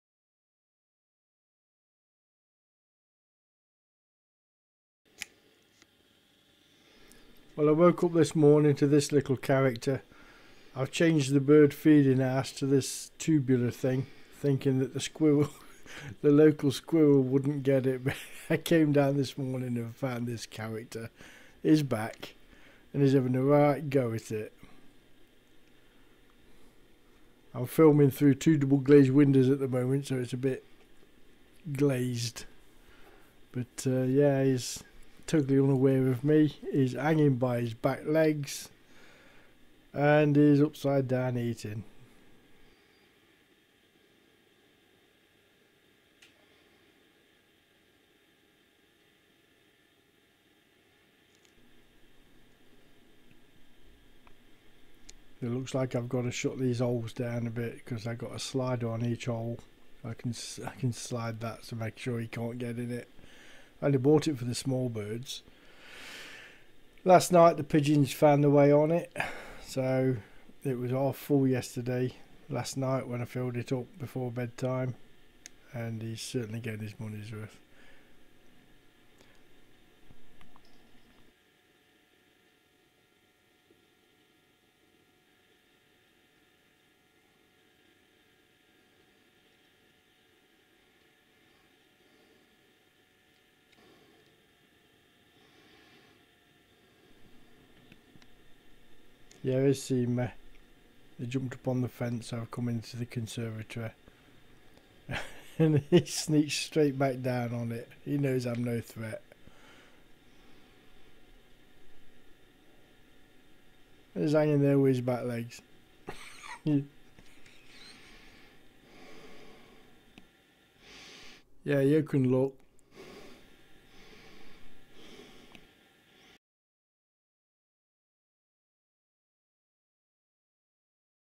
Mr Squirrel strikes again.
Mr. Squirrel has been a regular visitor to my bird table, so I changed it for a tube feeder. I didn't think Mr. Squirrel would be able to get to it but where there's a will. there's a way.